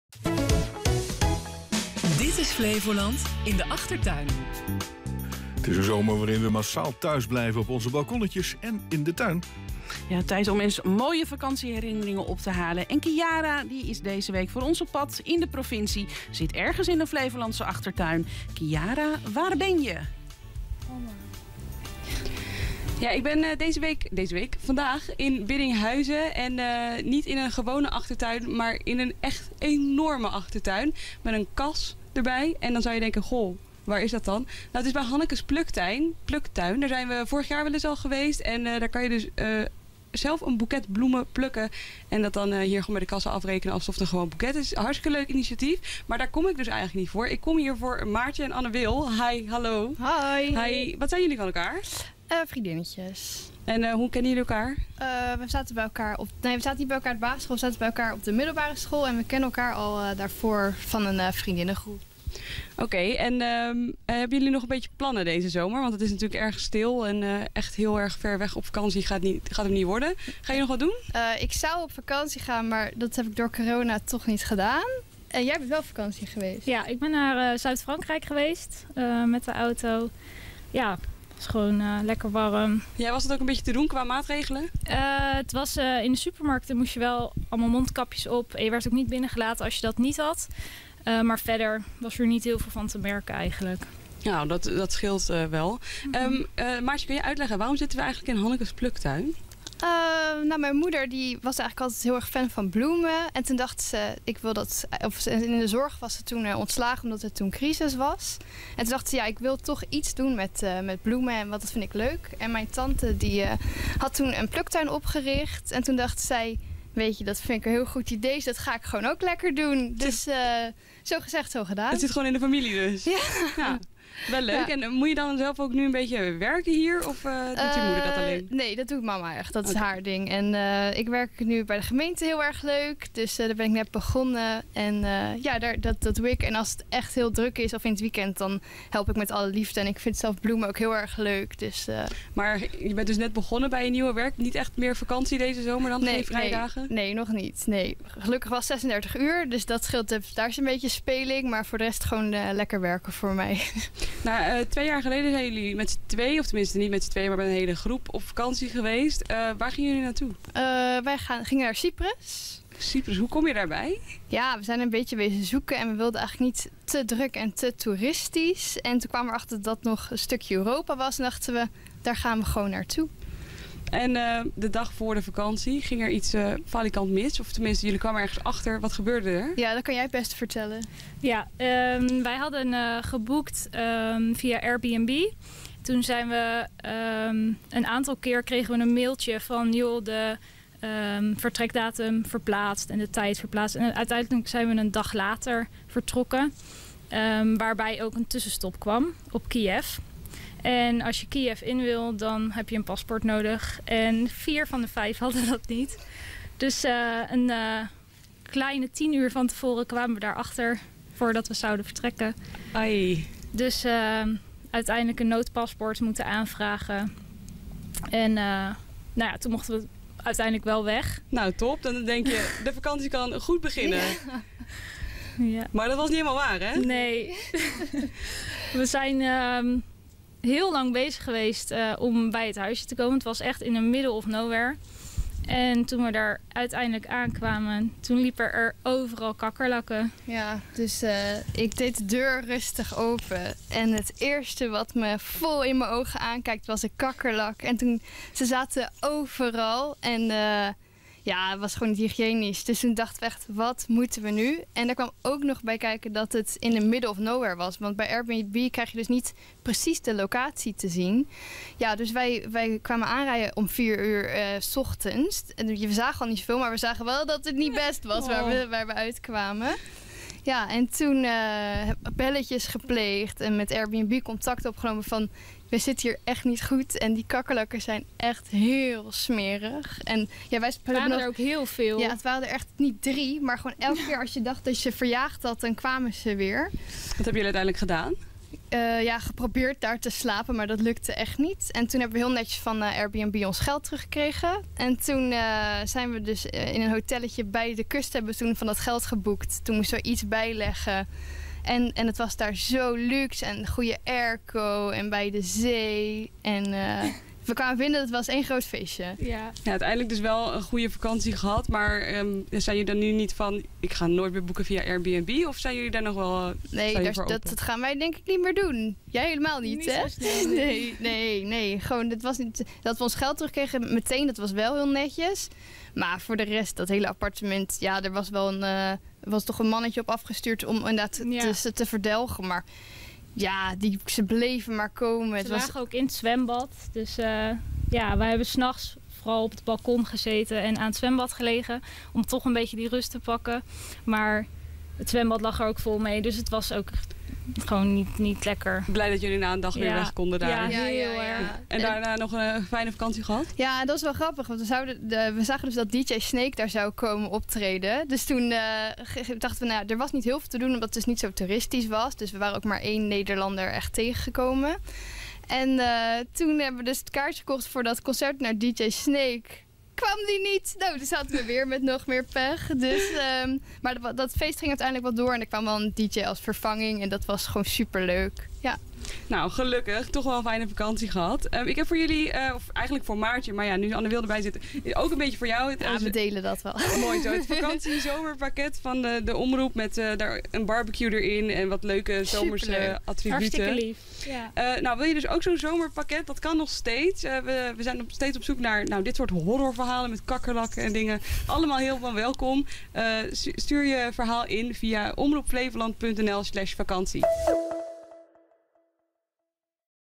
Kakkerlakken op Cyprus
In deze thuisblijfzomer halen we graag vakantieherinneringen op. Deze week vertelt Maartje over haar kakkerlak-vakantie op Cyprus...